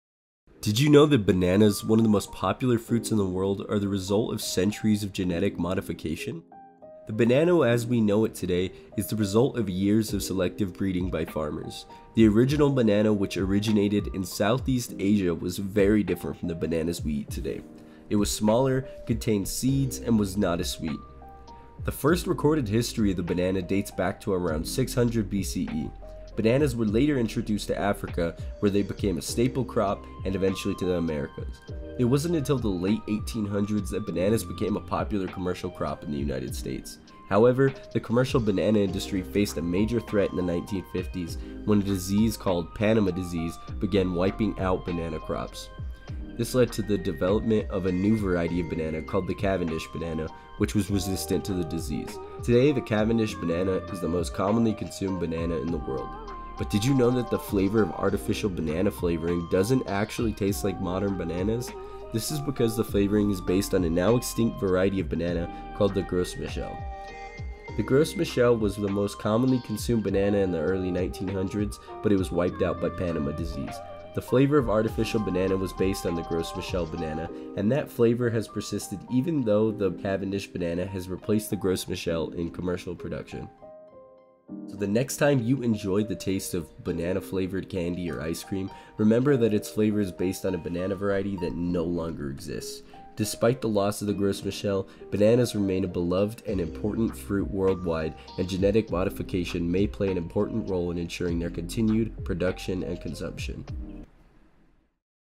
The Genetic Journey Of Bananas
I was challenged to make a video with the prompt *Old and New* as fast as possible. I decided to challenge myself additionally by doing the motion design for the map animation myself.